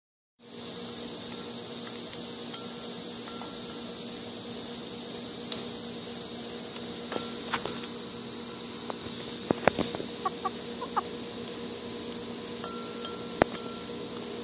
Hens enjoying yogurt
The white noise in the background is cicadas and other bugs. Yay Ohio summers.